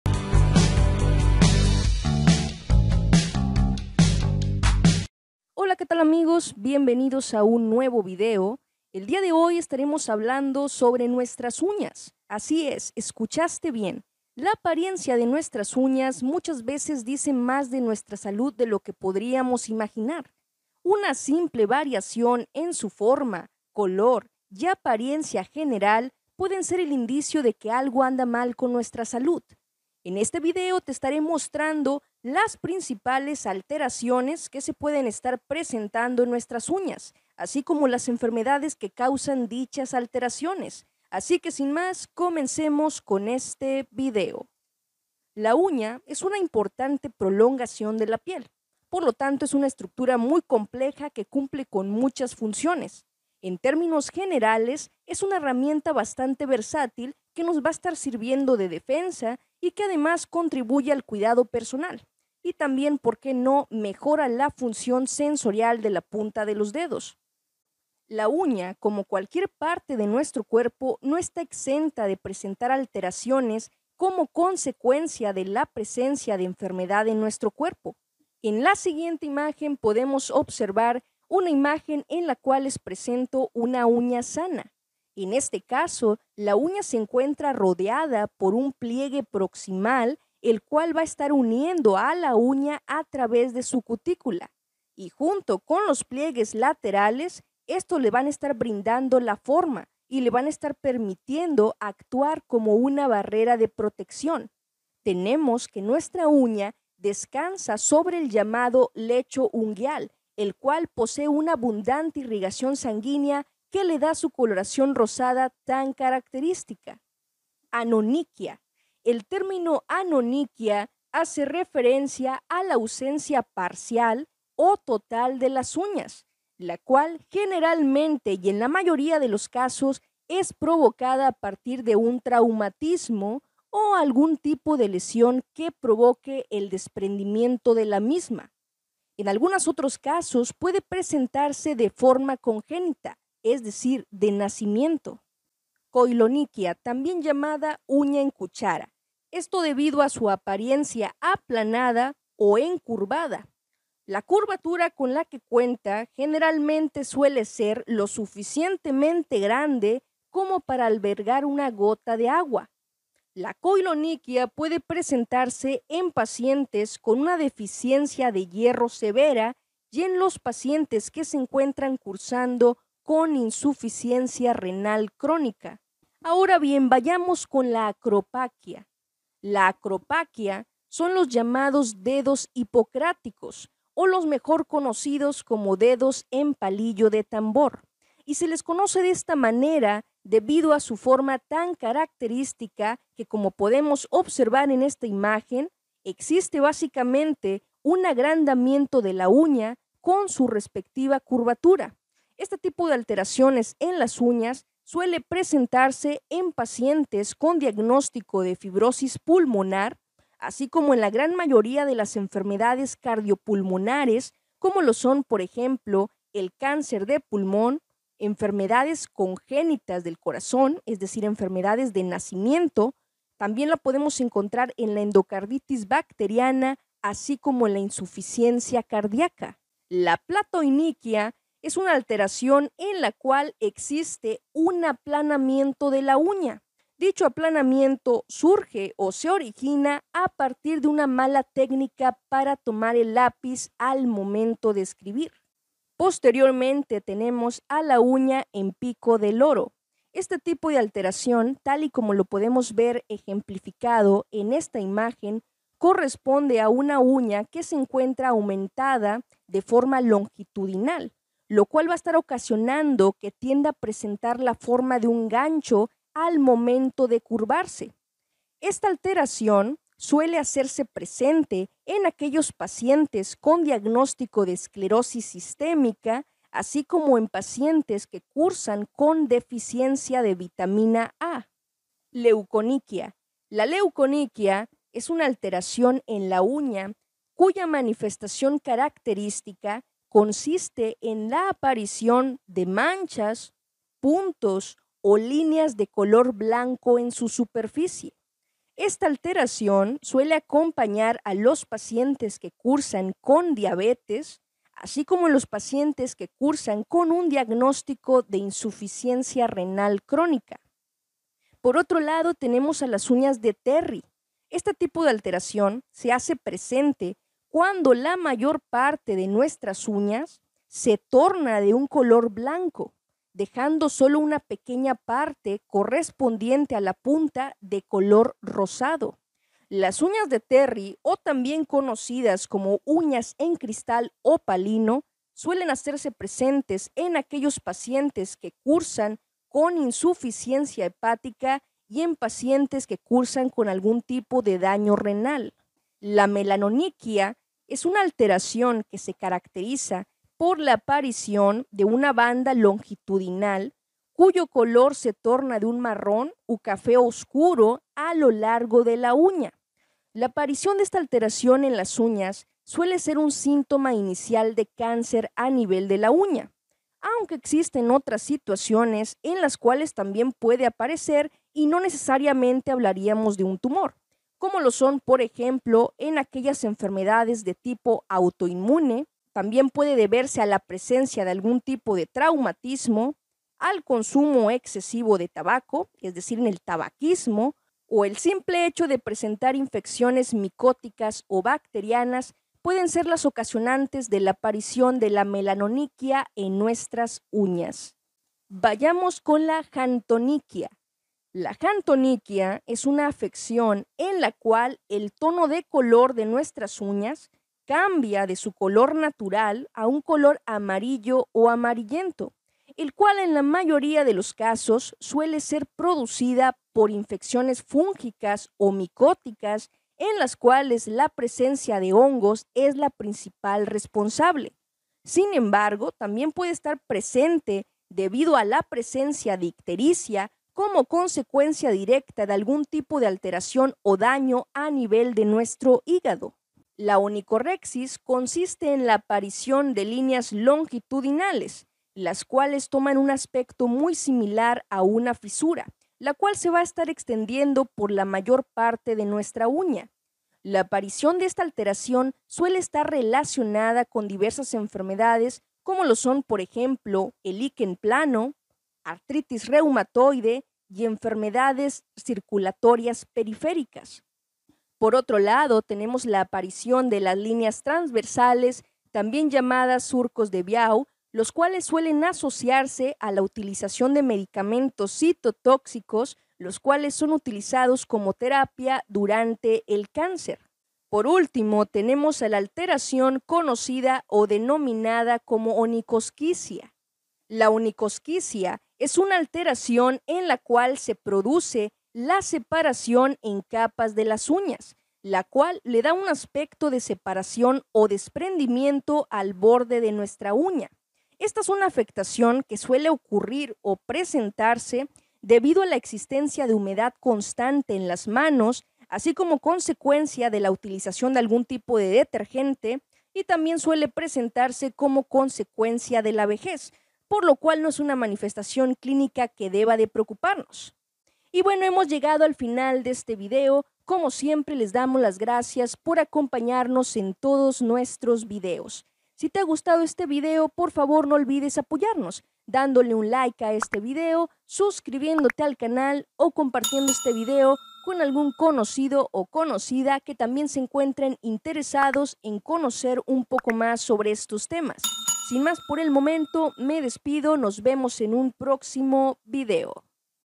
Cuidado ¿Porque salen lineas en las uñas? Lo que dicen tus uñas sobre tu salud - MEDIKA AC
Sabias que la apariencia general de tus uñas habla mas de tu salud de lo que te imaginas en este video te platicare sobre las principales alteraciones en las uñas asi como las enfermedades que ocasionan dichas alteraciones